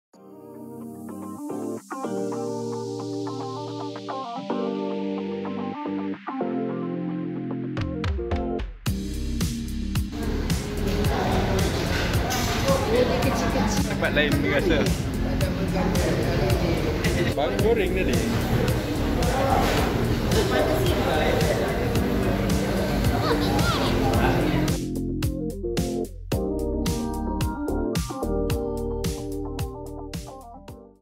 The Colours of Life - Reronda Cari Makan : Roti Puri, Seksyen 3, Bangi
The Colours of Life - Reronda Cari Makan : Edisi terobek lagi. Roti Puri, Seksyen 3, Bangi. Jenuh juga kami cari tempat makan lepas ngantor iqa. Hari ni kebetulan cuti. Maulidur Rasul. Banyak kedai yang tutup. Kami ronda aje. Sib baik kedai ni buka. Layan roti puri la apa lagi. Siap tambah lagi gitu. Best!